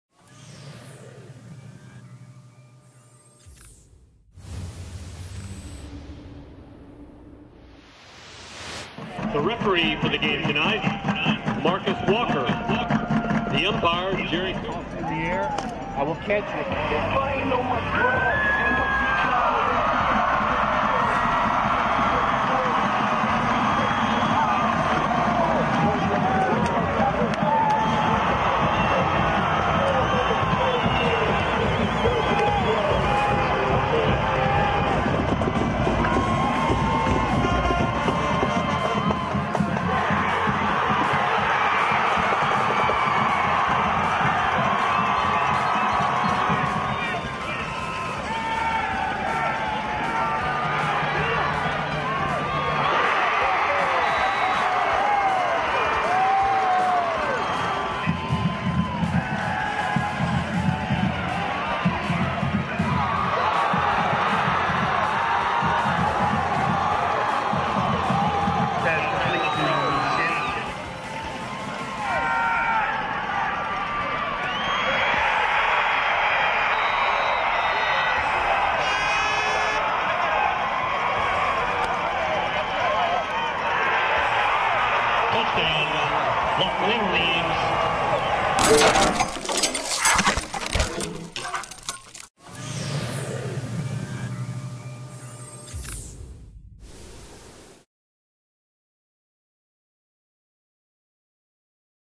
Jayquan Lincoln 11 Yard Touchdown Reception | 2016 Oklahoma High School Football
Jayquan Lincoln gets on the board first for Lawton in the 2016 6A II Championship game with an 11 yard touchdown catch.